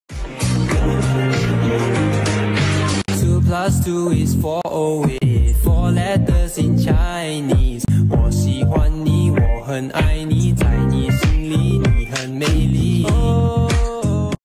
➤ • 4 letters in chinese !! 🇨🇳 • chilumi • genshin impact • gc • ᨎ
STOP IK I UPLOADED SO MANY GENSHIN VIDS I JS DONT KNOW WJICH FANDOM SHOULD I CHHOSE EVEN THO IM A MULTIFANDOM GACHATUBER ( its hard tbh )

inspo : @kit_610  ( i forgot to credit )

ship : chilumi

fandom : genshin impact

characters : lumine & tartaglia / childe